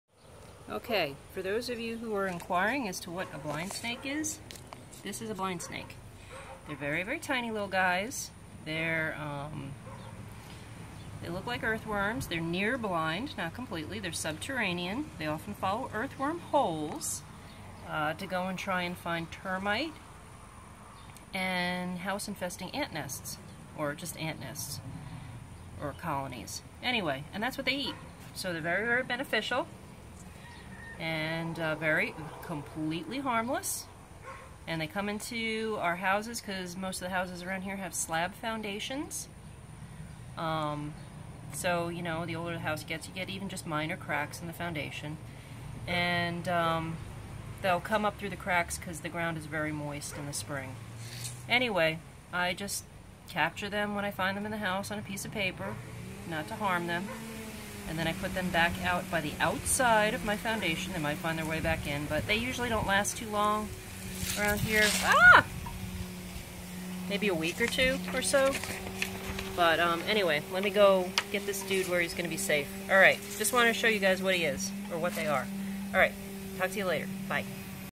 Texas Blind Snake
The Texas Blind Snake (or leptotyphlops dulcis) is a tiny, semi-blind, subterranean snake of the semi-arid Texas Gulf central coast & Mexican Gulf Coast, as well as the arid areas of the American southwest and Mexico.  It looks like a clean earthworm, & is a HARMLESS & BENEFICIAL little snake to have around your house or property, as the eat destructive termites and home-infesting ants!
They are NOT baby venomous snakes of ANY kind, & CANNOT bite you!  They are even immune to fire ants, & enough blind snakes can control huge colonies!  So these are our friends - please safe guard them, & DO NOT KILL THEM!!  If you find them on your house floor during the spring rainy season, then just gently simply scoop them up with a piece of paper (to not harm them!), & relocate them to the outside area of your foundation.